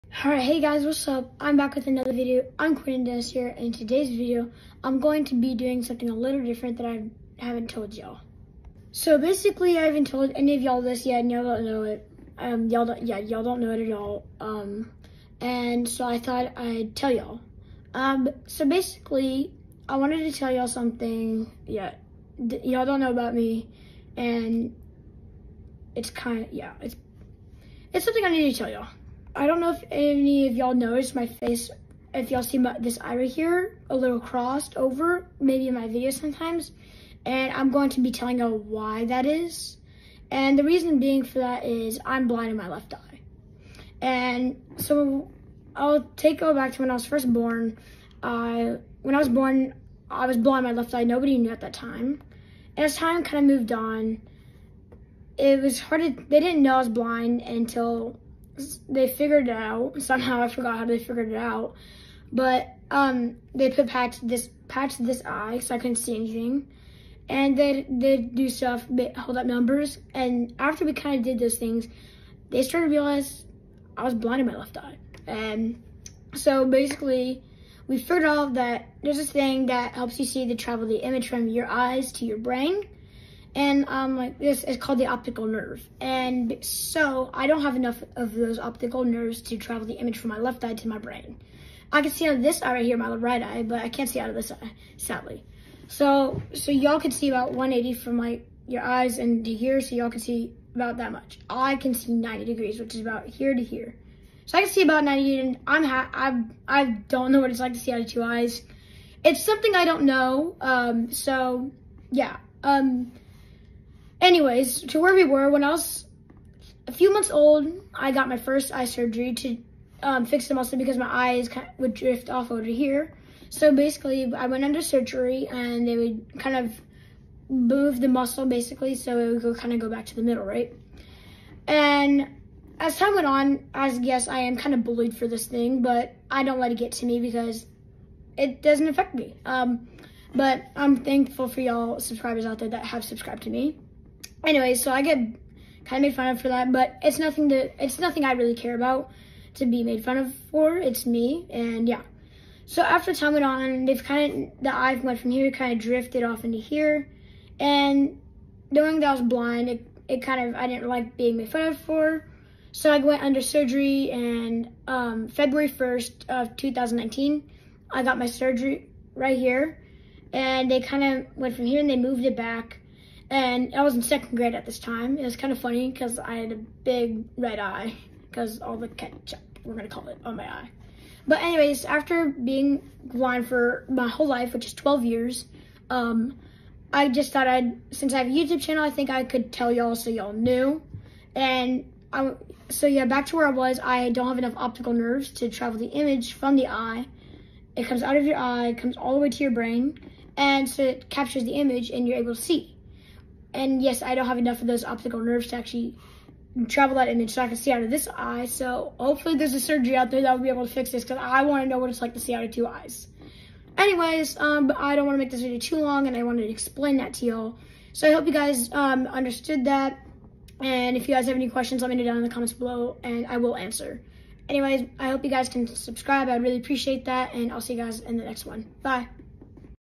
The truth about me!!
Hey guys in this video I explain something that’s personal in my life and that’s being blind in my left eye